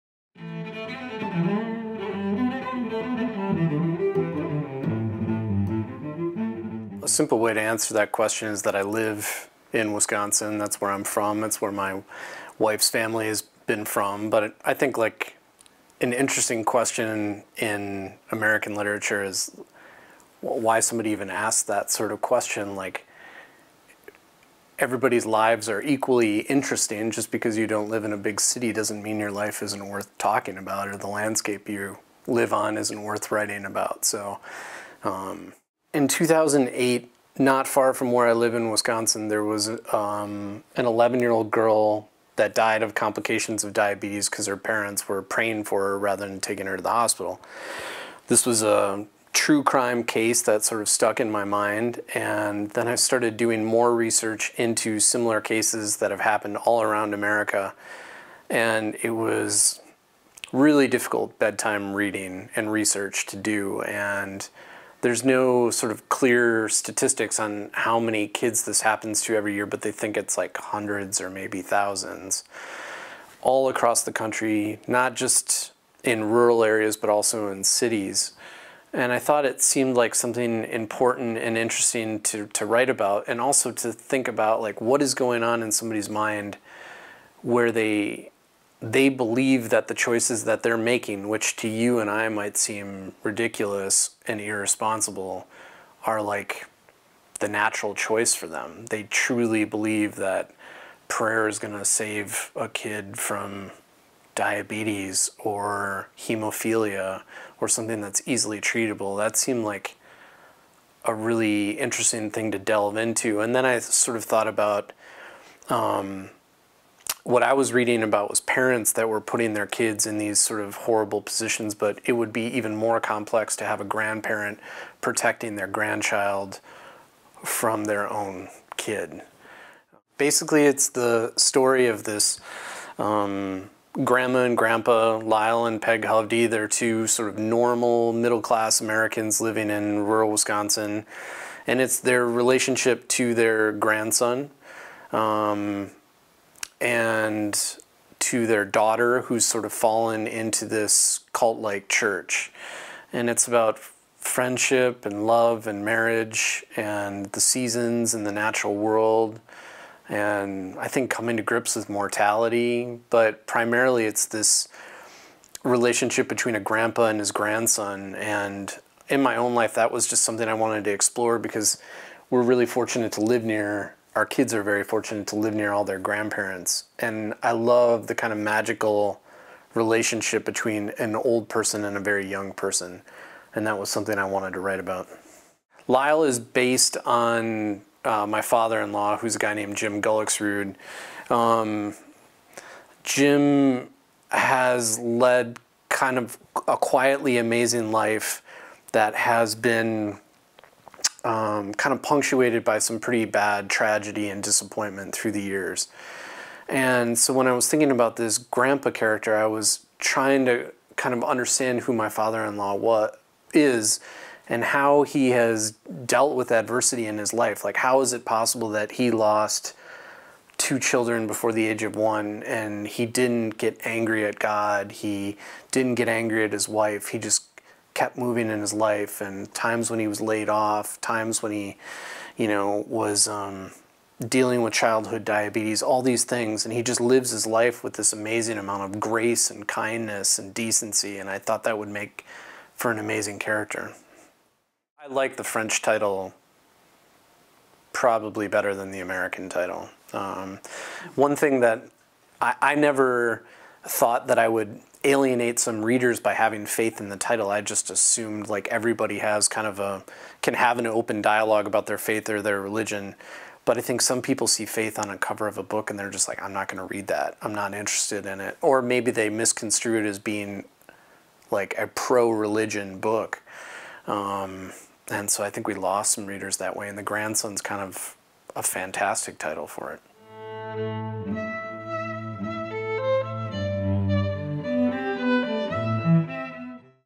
Nickolas Butler - Le petit-fils
Nickolas Butler nous présente son livre " Le petit-fils " aux éditions Stock. Rentrée littéraire janvier 2020.